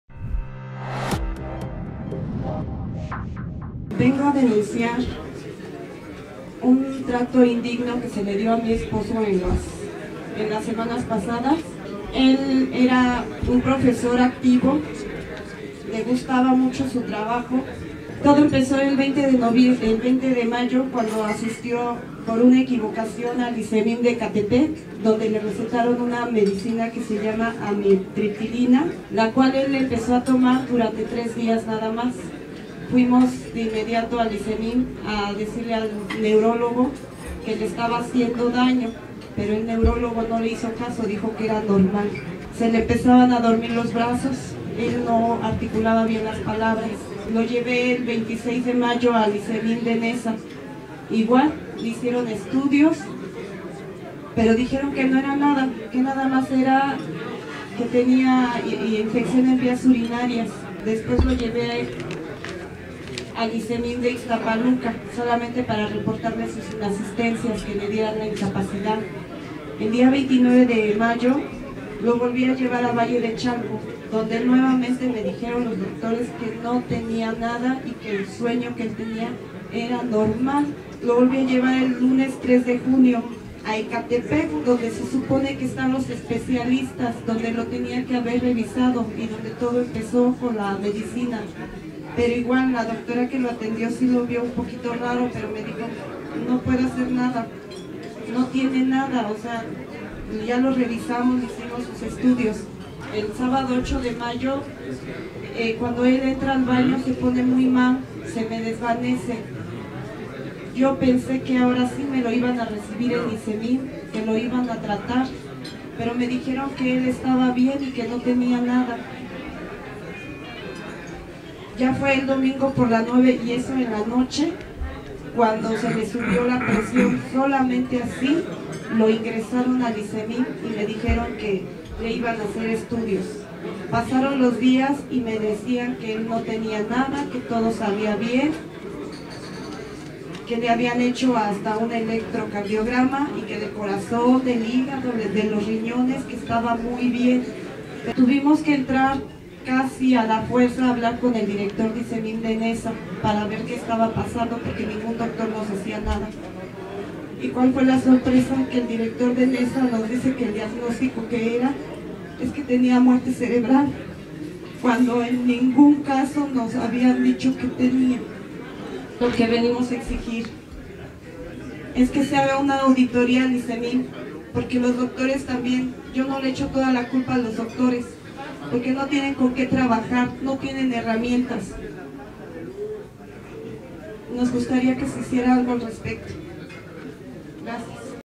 Viuda de profesor denuncia a Issemym Ecatepec por negligencia médica
Nidia Saraldea Ramos, profesora de la Primaría Juana de Asbaje, hace un llamado a las autoridades mexiquenses para revisar el caso de defunción de su esposo quien, desde finales de mayo pasado, por problemas de salud acudió a diversas clínicas del Issemym, en las cuales no se le hizo un diagnostico correcto por lo que finalmente murió.

El ahora occiso, Héctor Efrén González Saldívar también impartía clases en la misma institución educativa donde trabaja su esposa; la viuda denuncia que directivos del Issemym de Ecatepec de forma tardía y arbitraria le dijeron que su marido había fallecido por muerte cerebral.

Producción y edición: Angel Monroy